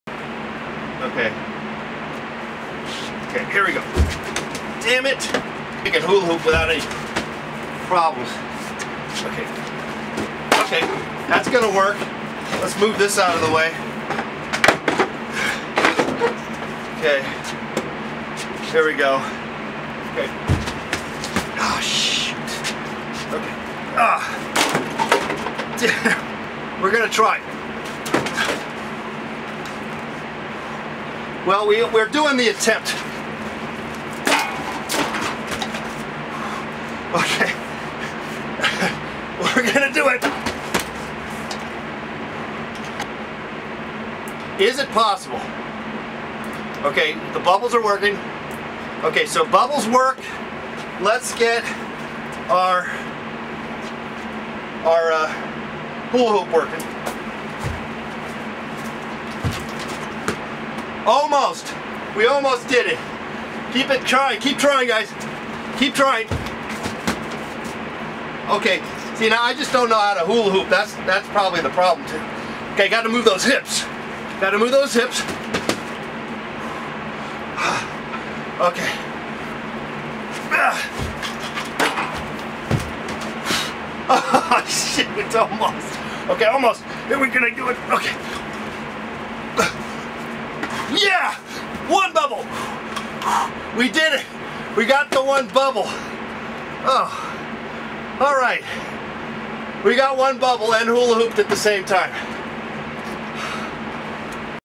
Let's Hula Hoop and Blow Bubbles TV
Mr Let's Paint tries to hula hoop and blow bubbles at the same time!